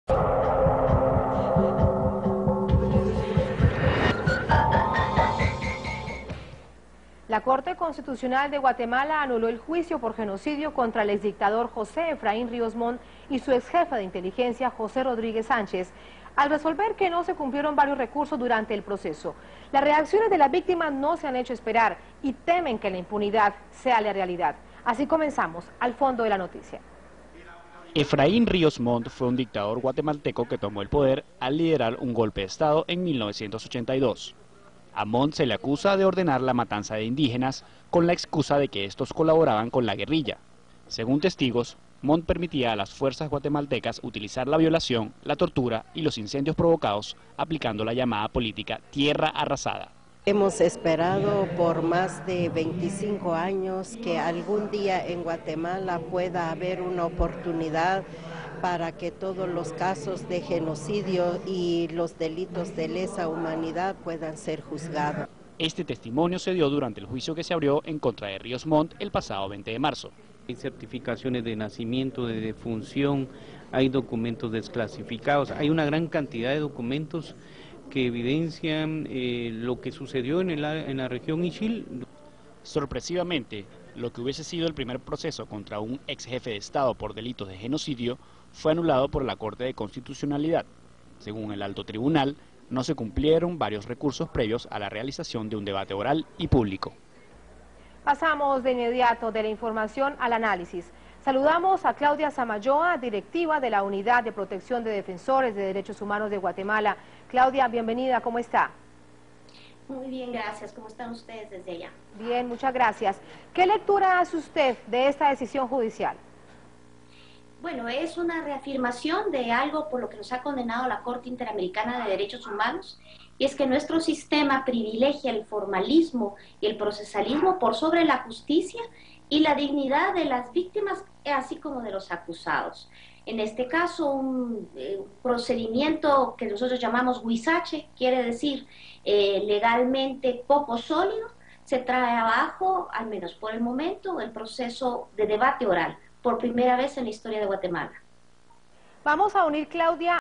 Guatemala: anulado julgamento contra Ríos Montt
A Corte Constitucional da Guatemala anulou o julgamento por genocídio contra o ex-ditador Ríos Montt argumentando que faltaram recursos durante o processo. A resposta dos familiares das vítimas foi imediato. Teme que depois de 25 anos, a impunidade reine no país. teleSUR